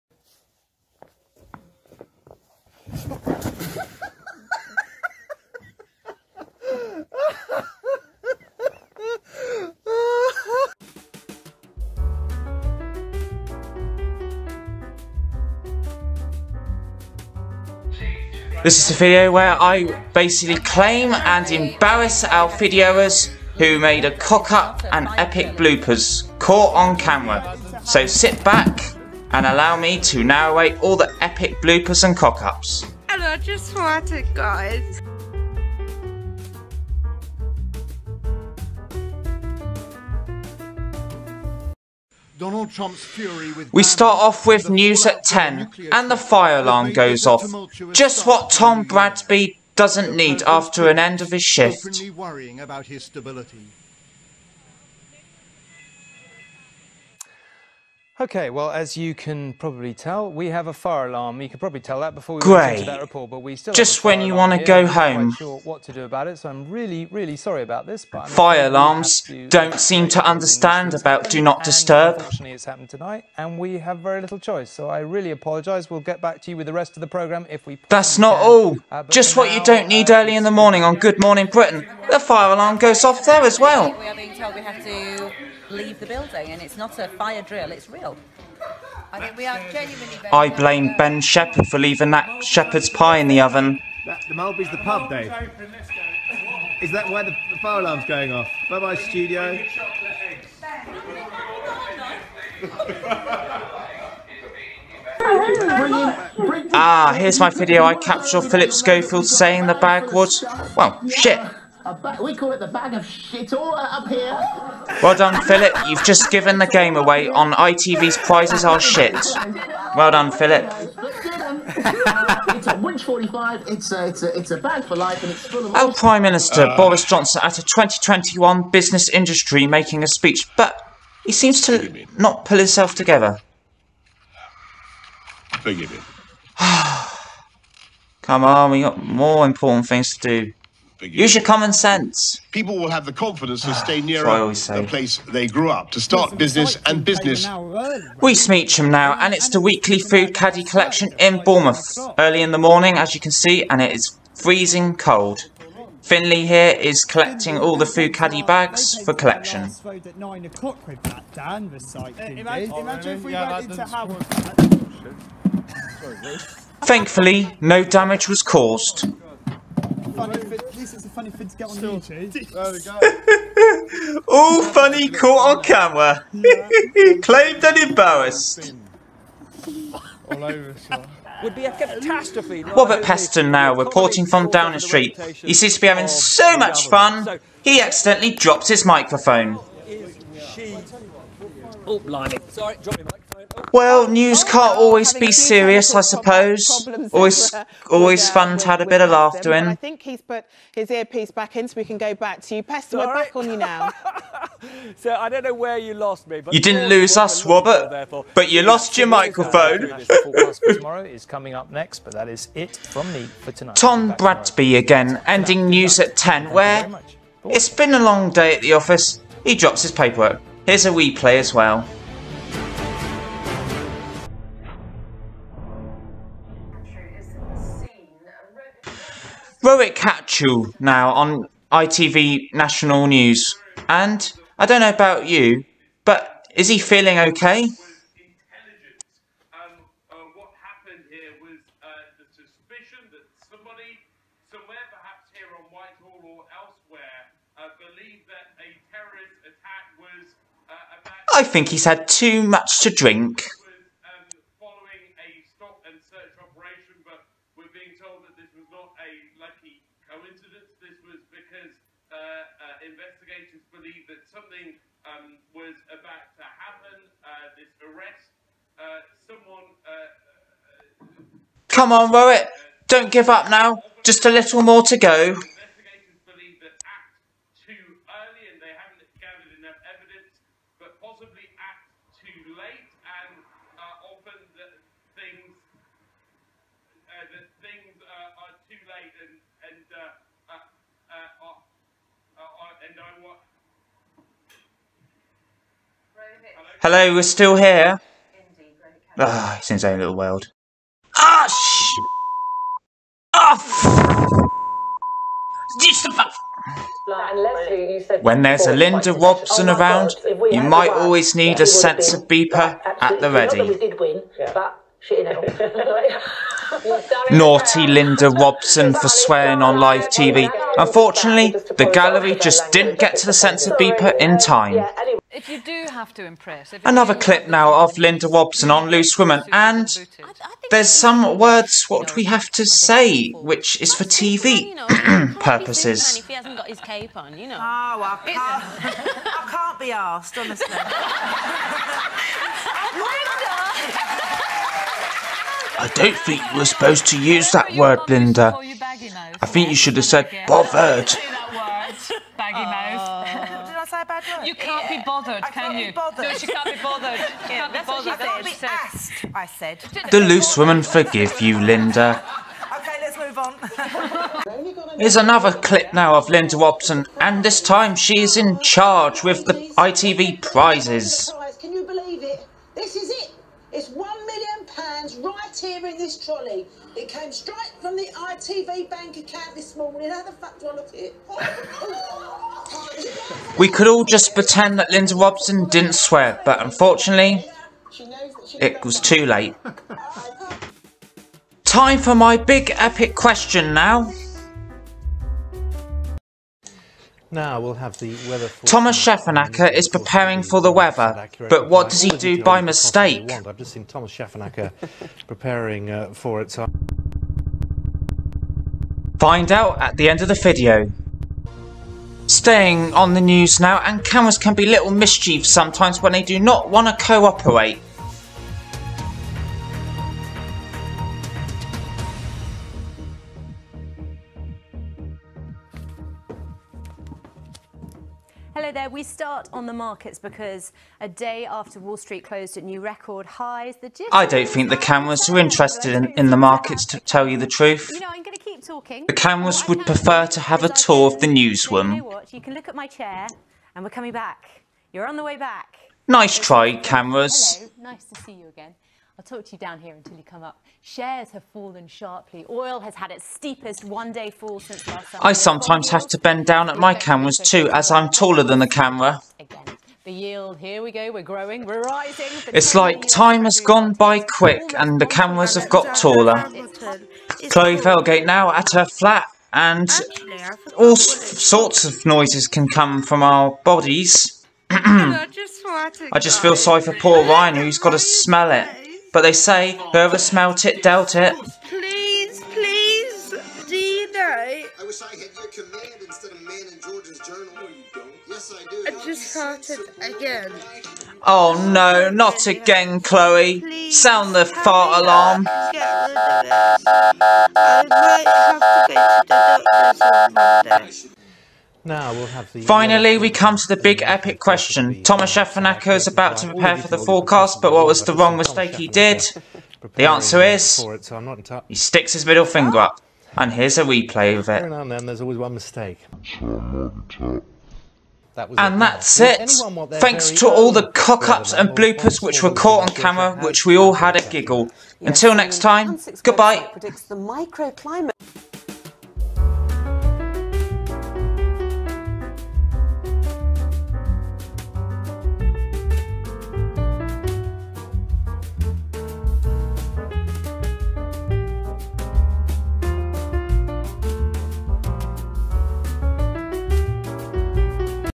Bradley Champion: narrating epic bloopers/cock-ups on TV and YouTubers
I'm narrating epic bloopers and cock-ups on TV and on YouTubers as well who made cock-ups and bloopers which made us all have a giggle behind our screens.